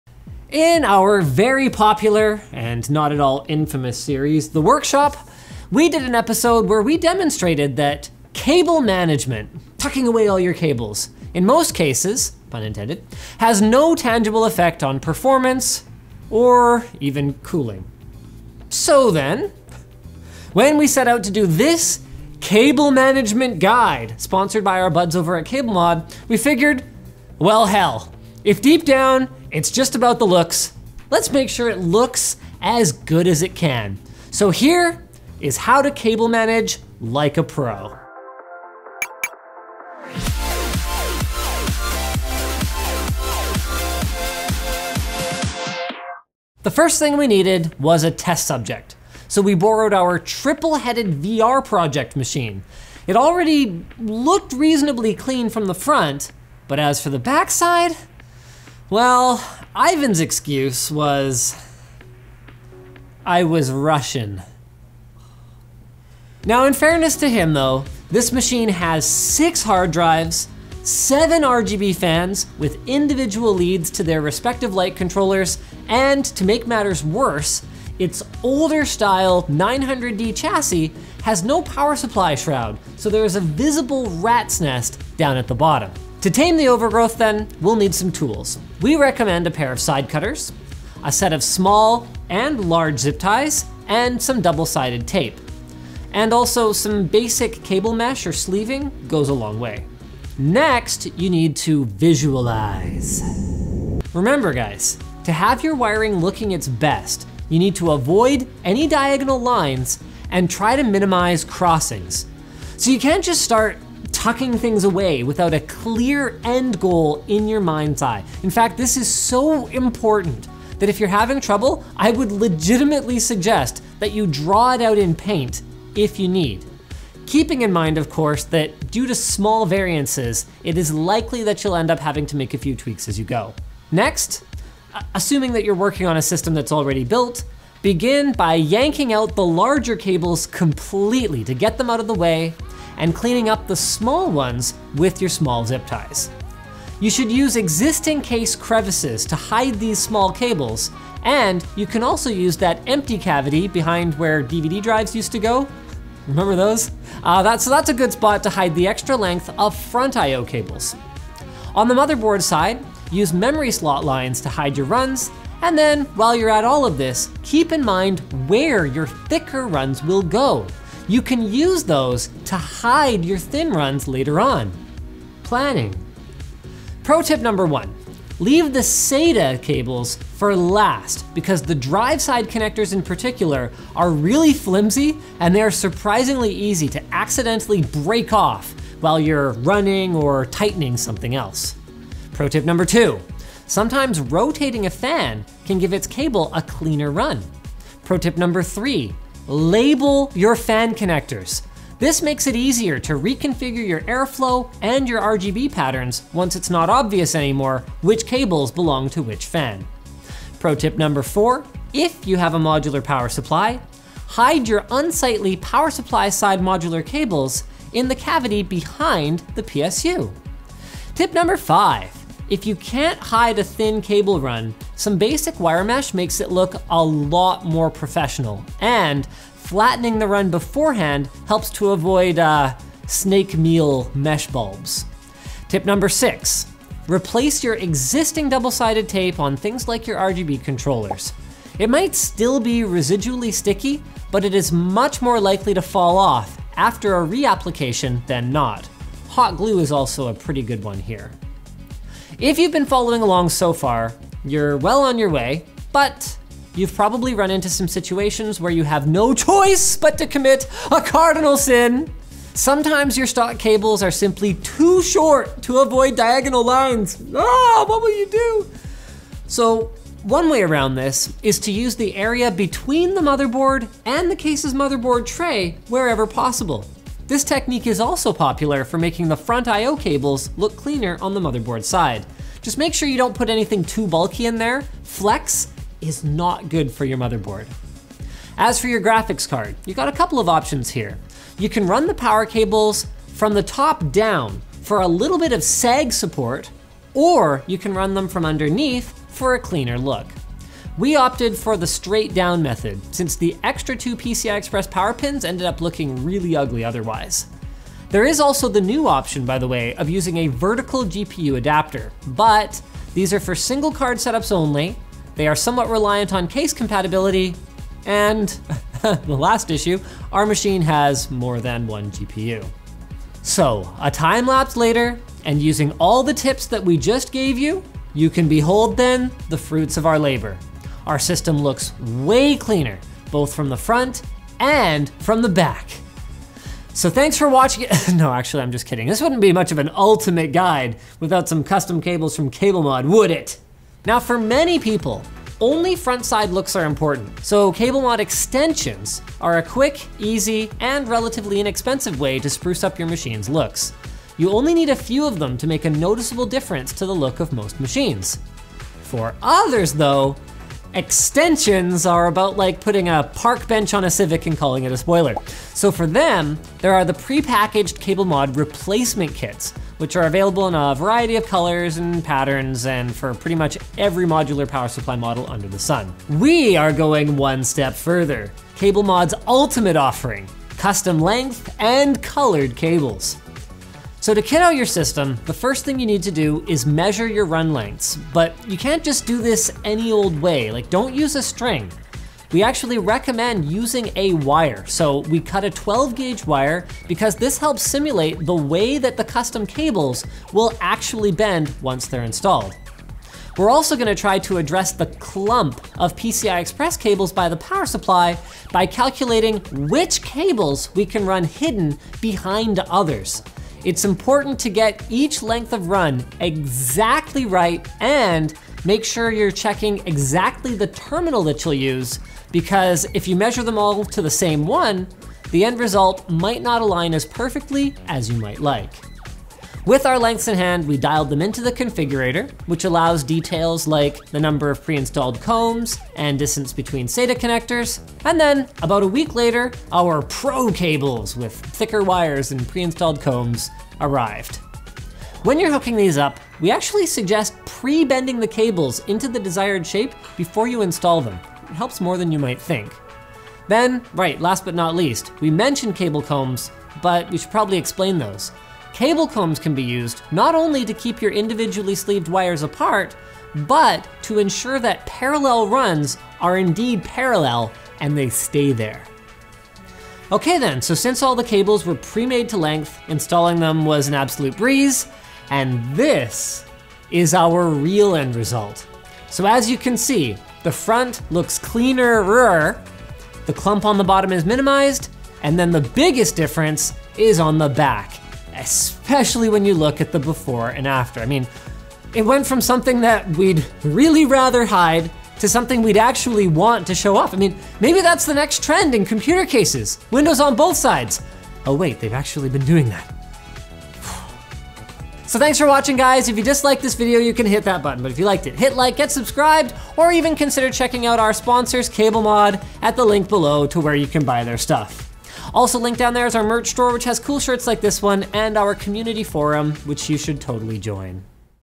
ULTIMATE Cable Management Guide
We go through the steps of taming one of the worst cabling messes like a PRO, custom-length cables and all.